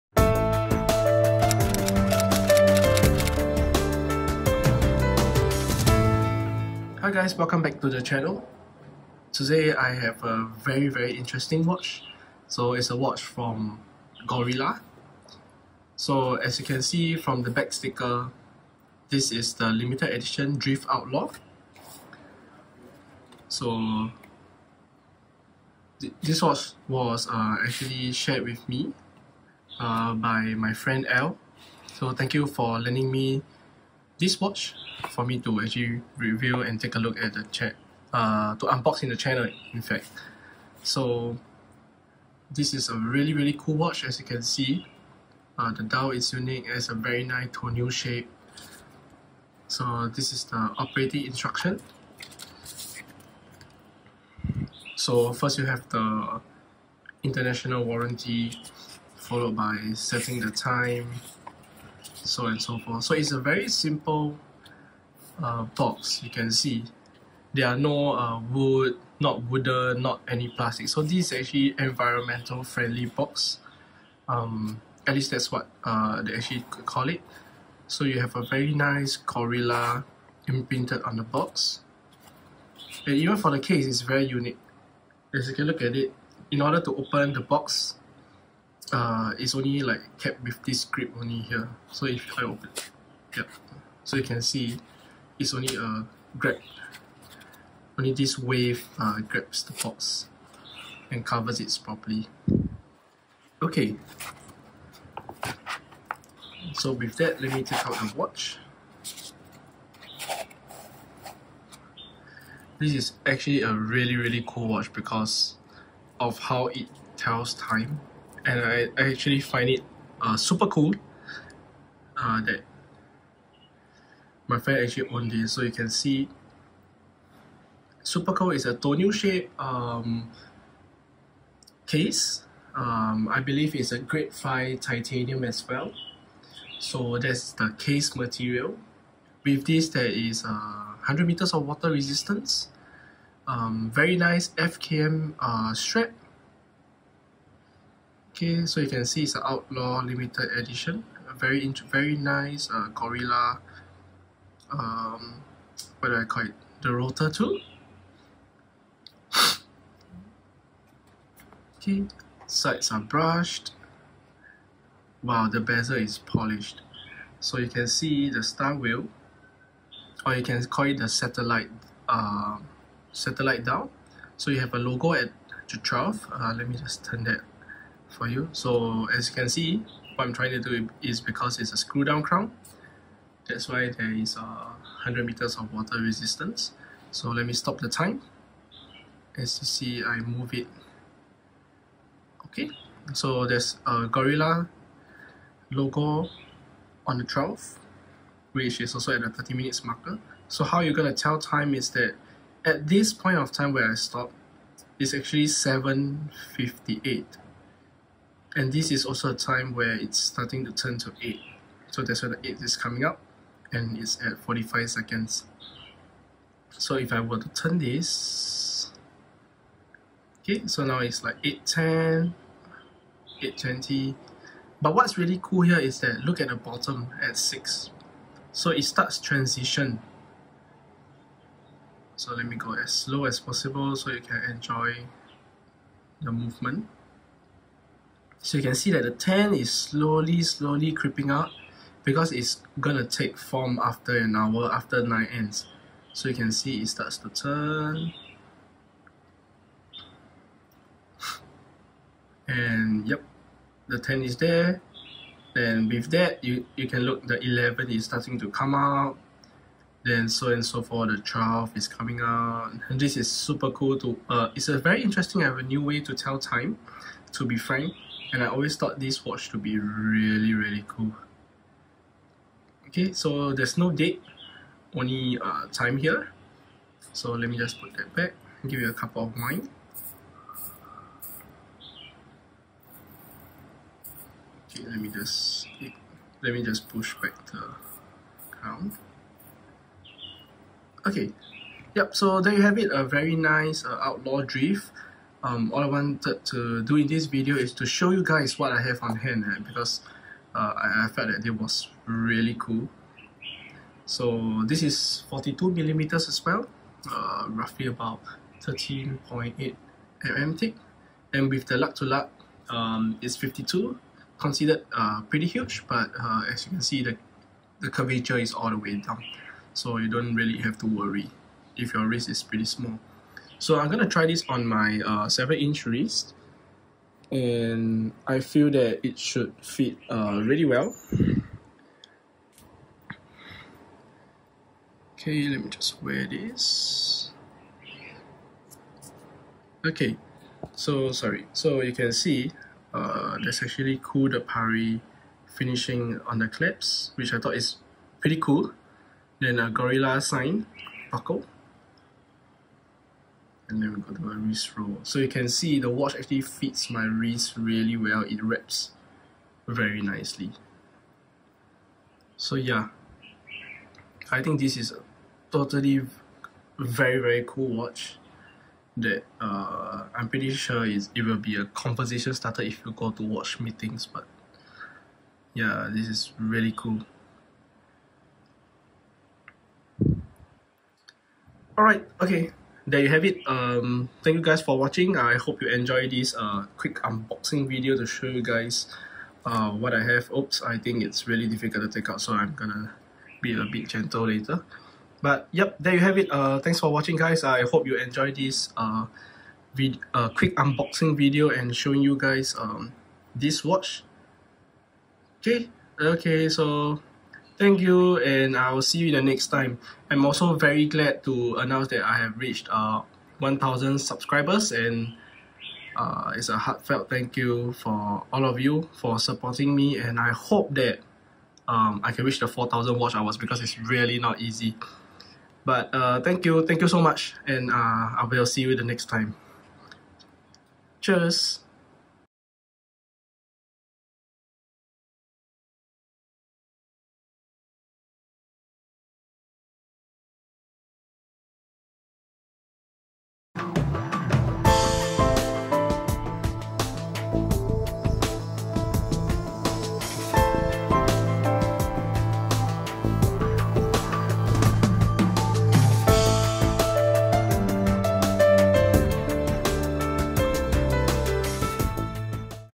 [Gorilla] Outlaw Drift
Hey there! Welcome back to the channel.

Today I'm sharing a watch where telling the time is done in a unique and super cool manner. Had it on my hands for the first time yesterday and I thought the watch to be super fascinating. I love how the wandering hours work when time passes. Surely, a piece that starts conversations and a special piece to own. Thanks to my friend Al for sharing this piece with me. Cheers!